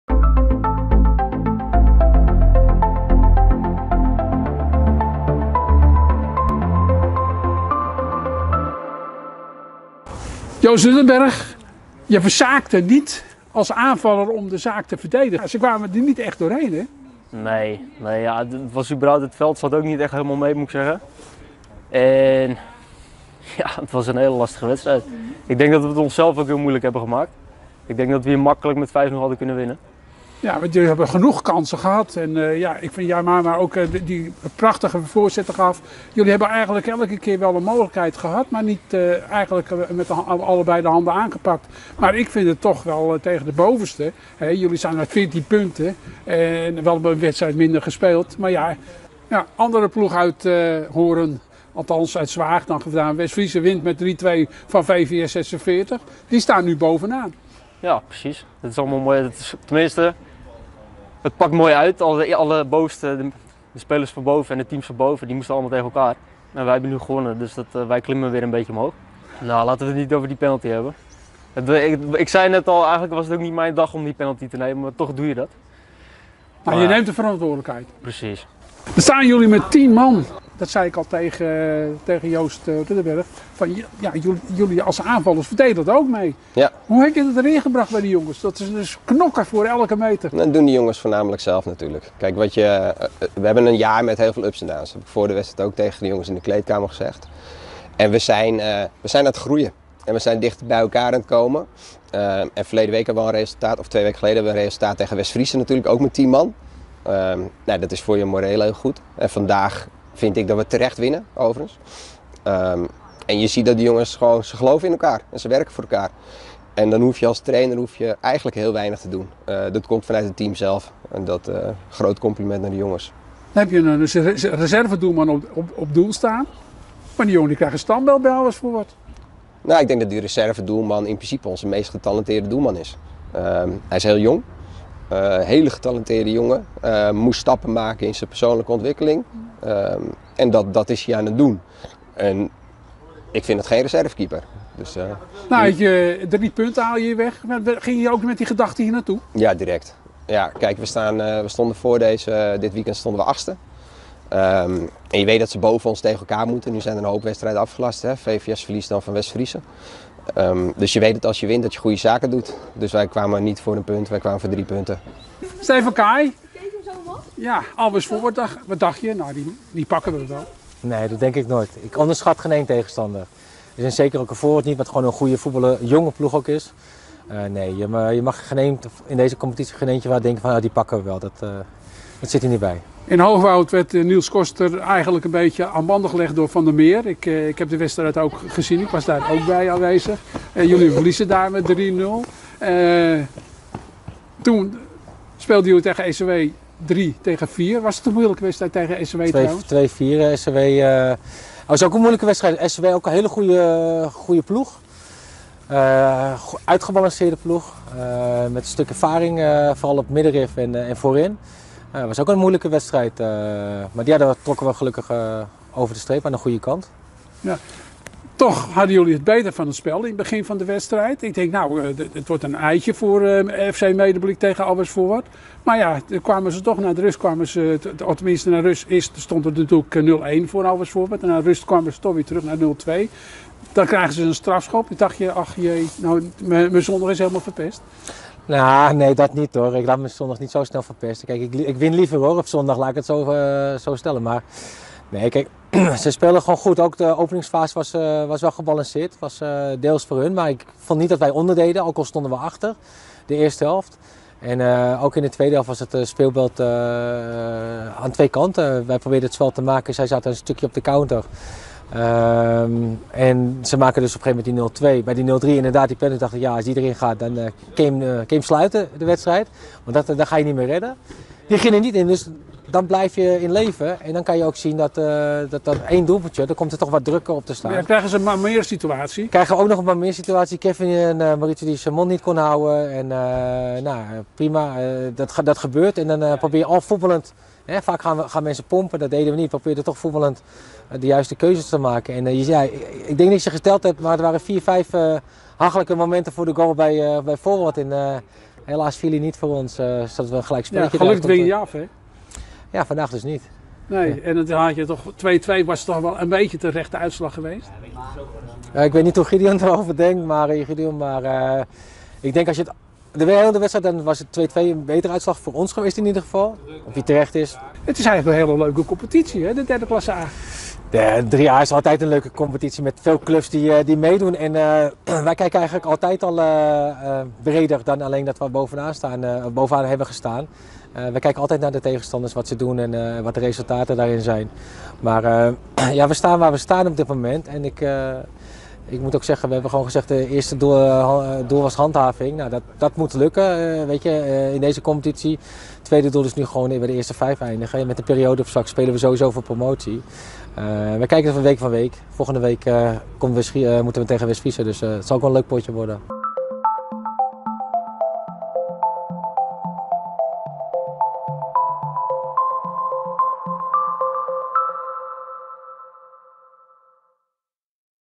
Always Forward verdient punten tegen koploper FC Medemblik
Mooi weer maar voor de toeschouwers was het koud. Toch zagen zij een enerverende wedstrijd van beide kanten.